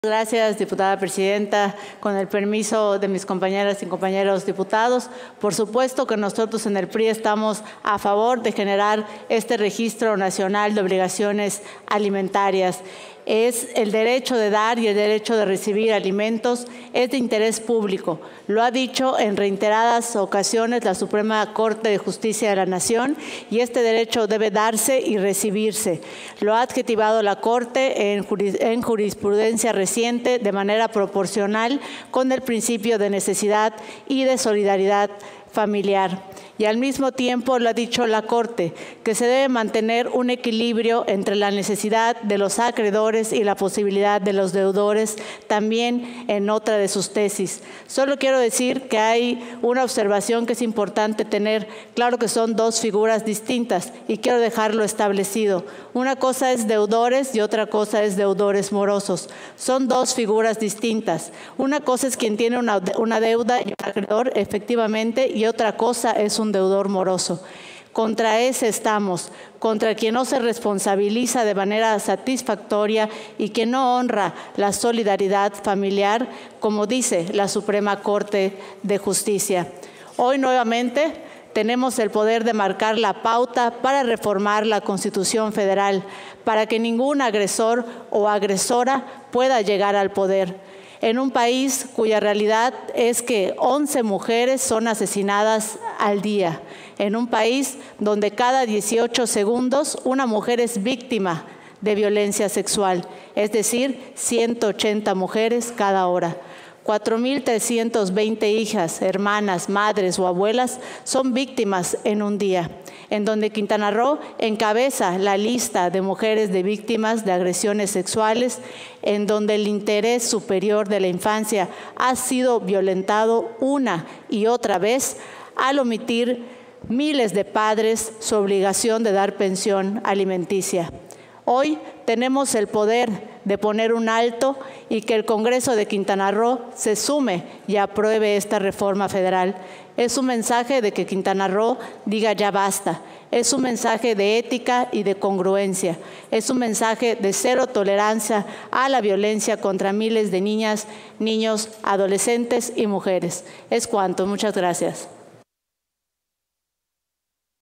Intervención Dip. Elda Candelaria Ayuso Achach. PRI (9/Mayo/2023)
Intervención de la Dip. Elda Candelaria Ayuso Achach, del PRI, en relación al Proyecto de Decreto por el que se reforman y adicionan los artículos 38 y 102 de la Constitución Política de los Estados Unidos Mexicanos, en materia de suspensión de derechos para ocupar cargo, empleo o comisión del servicio público; presentado por la Cámara de Senadores del H. Congreso de la Unión. Publicado en la Gaceta Parlamentaria Año I, número 53 Ordinaria, de fecha 9 de mayo de 2023.